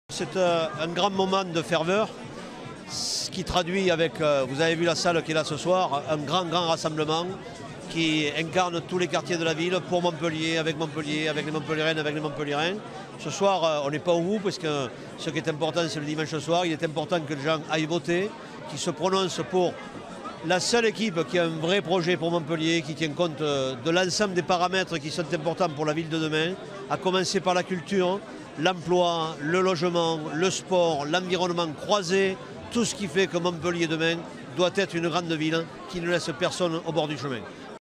Elections municipales : Le PS mobilisé pour le dernier meeting de son candidat à Montpellier
Près de 500 personnes étaient rassemblées pour l'ultime meeting de Jean-Pierre Moure, avant le premier tour de l'élection municipale, mercredi 19 mars 2014, salle Nogaret à Montpellier (34). Au premier rang, le maire sortant Hélène Mandroux, le patro